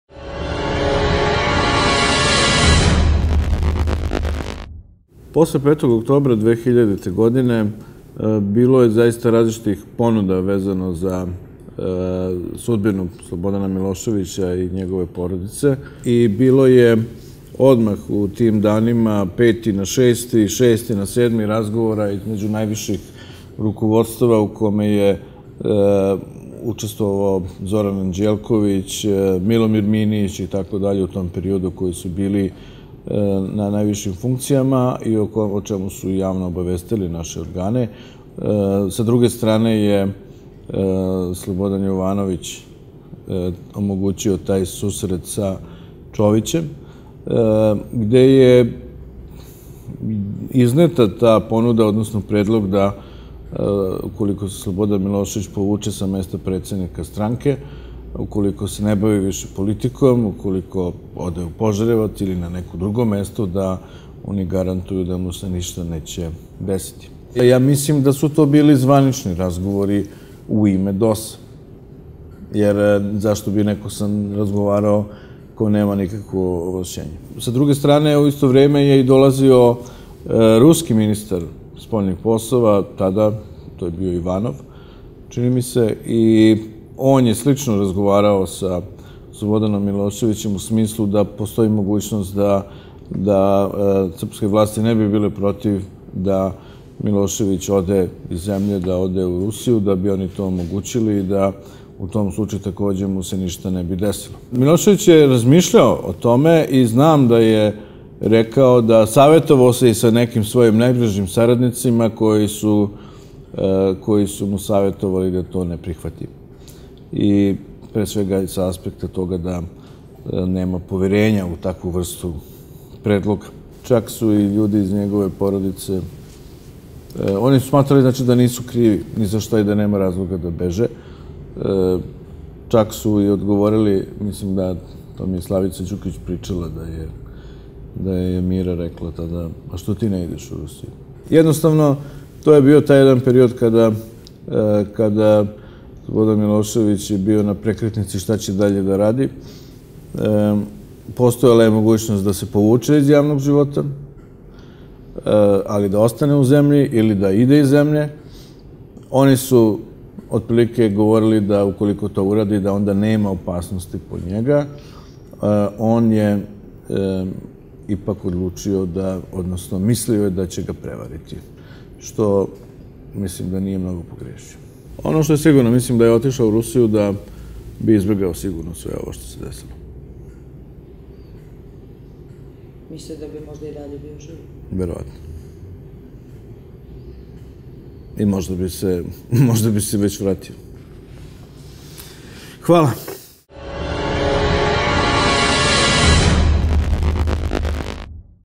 ISTRAŽIVAČKI BIRO Dačić ekskluzivno: DOS hteo Slobu da skloni u Moskvu
Pratite nas i na Viberu ili Vajberu, kako se kome više sviđa

ZAVERA: Plan tadašnje vlasti bio da Miloševića zaštite od optužnice za ratni zločin i skinu odgovornost sa sebe. U ime DOS tajne pregovore vodio Nebojša Čović, a predstavnik SPS bio Zoran Baki Anđelković

BEOGRAD - Vlada DOS pokušala je da spase bivšeg predsednika SRJ Slobodana Miloševića iste noći kada ga je svrgla s vlasti, oktobra 2000., ponudivši mu da ga sklone u Moskvu, gde mu „niko ne bi mogao ništa“! Posle punih 16 godina zataškavanja, ova najmračnija tajna 5. oktobra konačno je otkrivena, a o svim detaljima ovog „tajnog dila“, ko su bili pregovarači i koja je u svemu tome bila uloga velikih sila za Kurir ekskluzivno svedoči Ivica Dačić, tada portparol SPS.

Iako je narod bio željan pravde koju mu je DOS obećao, tvrdeći da će Milošević i njegova porodica odgovarati pred sudom za sva navodna nedela koja su počinili, čelnici nove vlasti u tajnosti su pregovarali sa bivšim predsednikom SRJ i sa Rusijom!

Odbio ponudu

- U danima posle 5. oktobra, peti na šesti, sedmi oktobar, došlo je i do razgovora u kojima su u ime SPS učestvovali Zoran Baki Anđelković i Milomir Minić. Sastanak je omogućio Slobodan Jovanović (šef beogradskog SPS tokom devedesetih godina). Predstavnik DOS bio je Nebojša Čović. Jedna od ponuda DOS, odnosno predlog, bio je da mu, ukoliko se Milošević povuče sa mesta predsednika stranke, ako prekine da se bavi politikom, ukoliko ode u Požarevac ili na „neko drugo mesto“, oni garantuju da mu se ništa neće desiti.

Pretpostavljam da je Čović bio izaslanik i da je o predlogu postojala saglasnost celog DOS. U isto vreme, u Beograd je dolazio ruski ministar spoljnih poslova, tada to je bio Igor Ivanov. On je takođe razgovarao sa Miloševićem na sličnu temu. On mu je preneo da postoji mogućnost da srpske vlasti ne bi bile protiv toga da ode iz zemlje, da ode u Rusiju, da bi oni to omogućili i da mu se u tom slučaju ništa ne bi desilo. Milošević je o svemu tome pričao na našim koordinacijama, a o tome su pričali našli rukovodioci u tom periodu - priča Dačić.

Na pitanje da li je Milošević takvu ponudu odmah odbio, Dačić odgovara:

- Milošević je razmišljao o tome. Rekao je da je pričao i sa nekim svojim najbližim saradnicima i ljudima iz bezbednosti, koji su mu savetovali da to ne prihvati jer nemaju poverenja u takvu vrstu predloga. Rekao nam je da je sve te predloge odbio, da neće da ide iz zemlje, čak nije hteo ni da se povuče s mesta predsednika SPS.

Na pitanje zašto je tadašnji DOS uopšte došao na ideju da Miloševića prebaci u Rusiju, Dačić odgovara:

- Plašili su se Miloševića u Srbiji, aktivnog Miloševića, jer ni tada još nije bila čista situacija. Plašili su se reakcija, a znali su i za obaveze koje će imati pred Haškim tribunalom. Pretpostavljam da je i to bio razlog. Da je Milošević otišao u Rusiju, Srbija ne bi imala više nikakvu vrstu odgovornosti, pritisaka da on bude uhapšen, izručen.

Na pitanje da li bi Milošević bio živ da je došlo do takvog scenarija, Dačić kaže:

Još bi bio živ

- Verovatno. I možda bi se već vratio.

Podsetimo, bivši predsednik Srbije uhapšen je aprila 2001. godine u Beogradu, a 28. juna, na Vidovdan, preko Tuzle je avionom prebačen u Haški tribunal. Umro je 11. marta 2006. godine u pritvorskoj jedinici Haškog tribunala, ne dočekavši kraj suđenja.